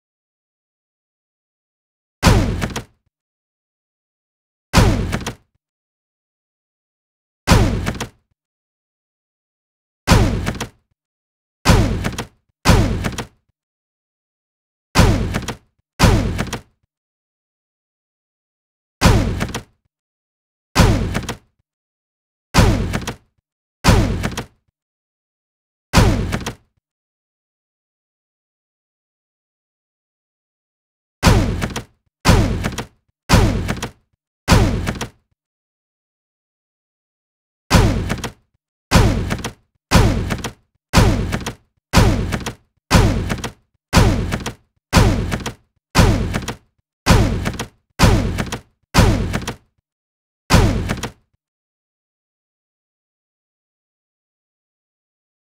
Ludum Dare 50 - Progress 1
Finally have some gameplay setup, but it was after a lot of soul searching and foundational coding. Time to make some L E V E L S